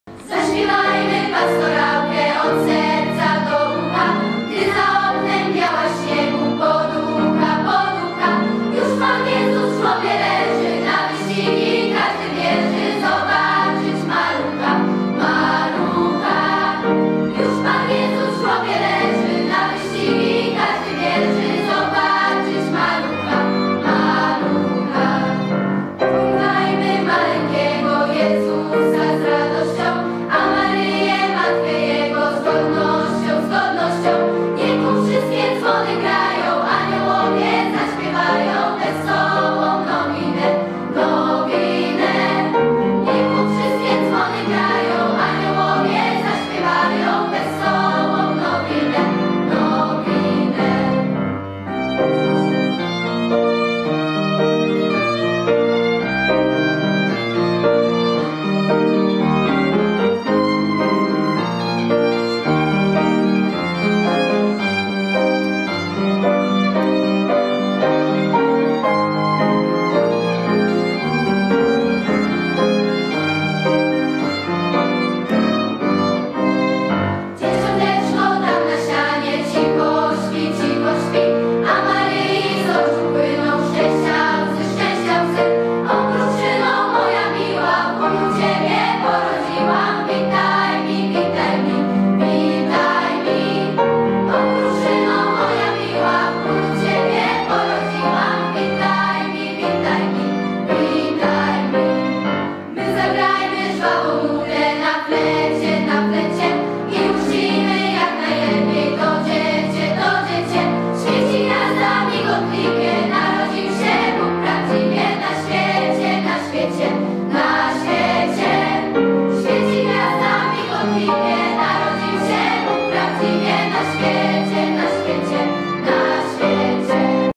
"Pastorałka od serca do ucha " - koncert kolęd - 31 I 2019 PSM I st. ZPSM w Jarosławiu
Chór PSM I st. ZPSM w Jarosławiu